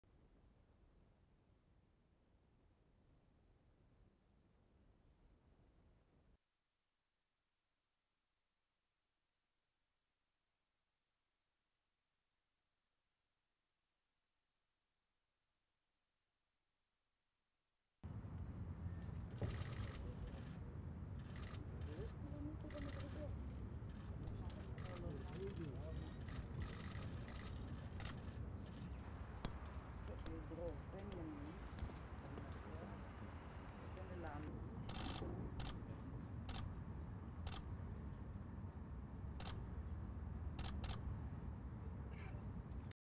PM Netanyahu Meets with Chinese PM - Part 1
Prime Minister Benjamin Netanyahu Meets with Chinese Prime Minister Li Keqiang

Video: GPO